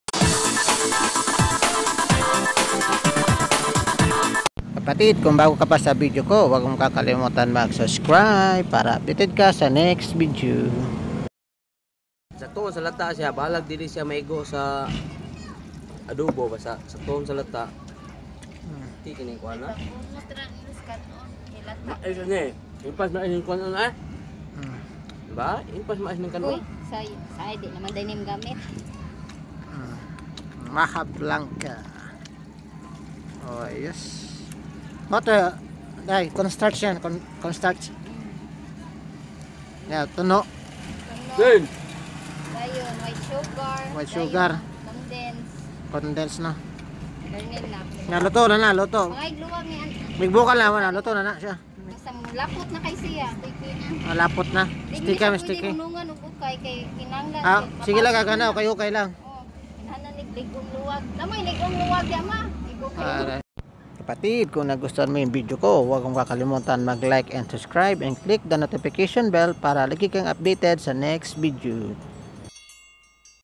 MAJA BLANCA WOW SARAP..PAANO ANG TAMANG PAGLULUTO NITO PARA LALONG SUMARAP.
JAYPEE TUKLAS TV